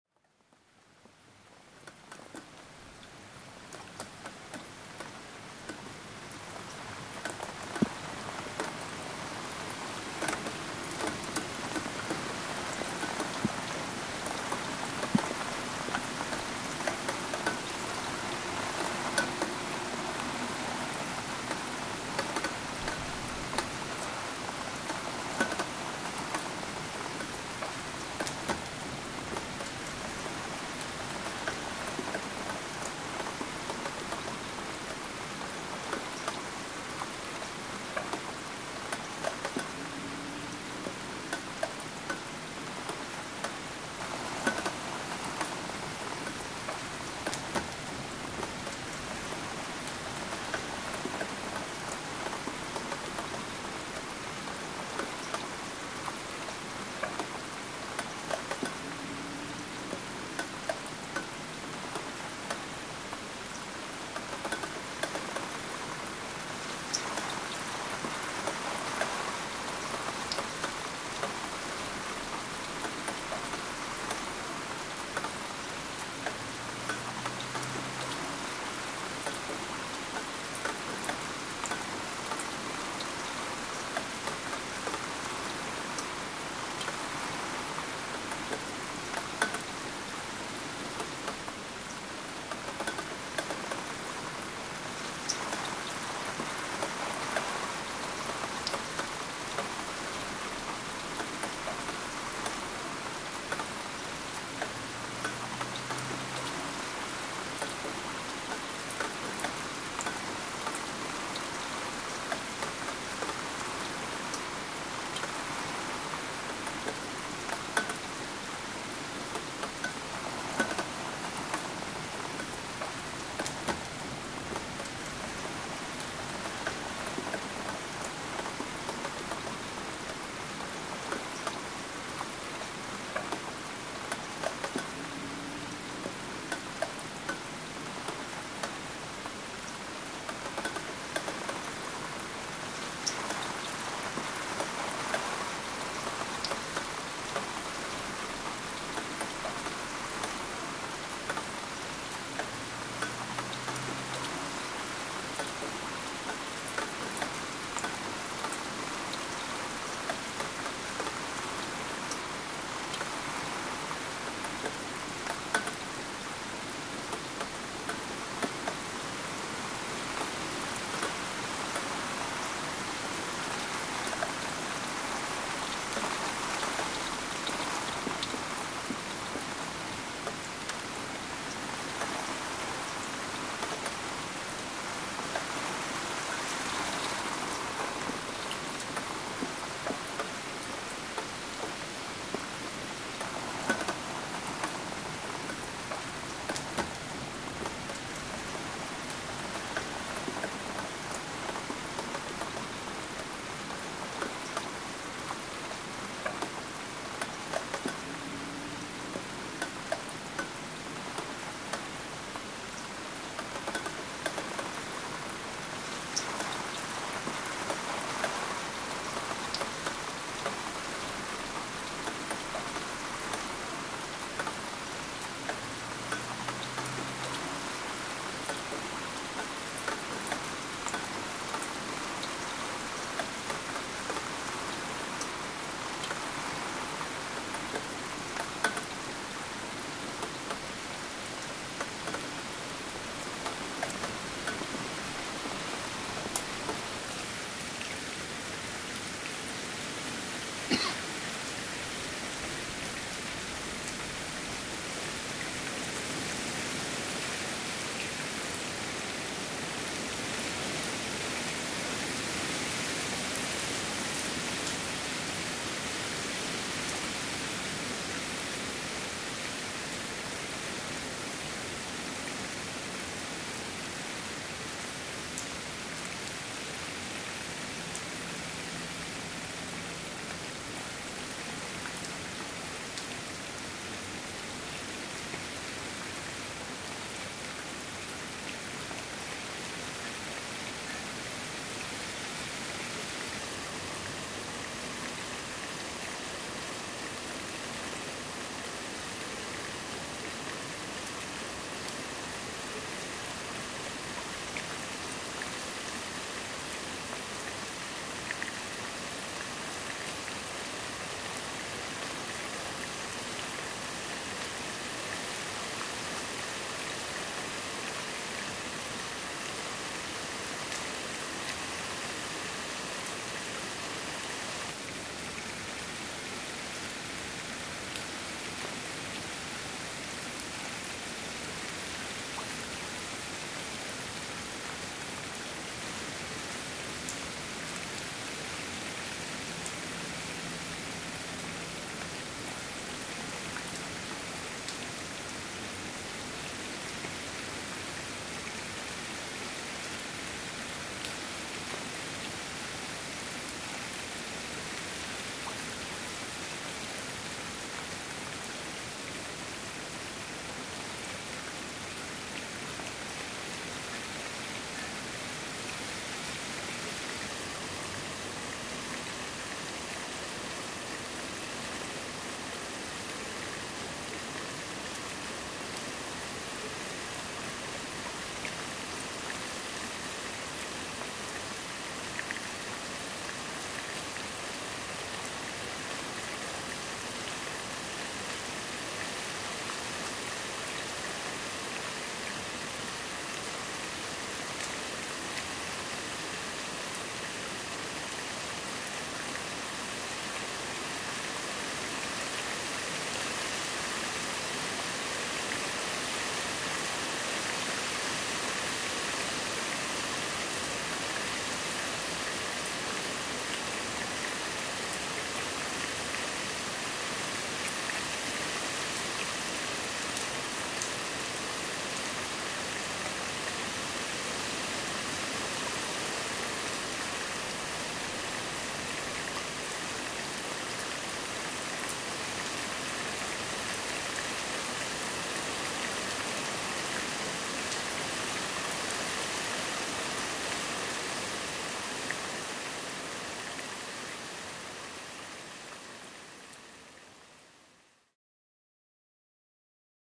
Regen in Oktober
Een eigen opname van actuele regenval op mijn balkon.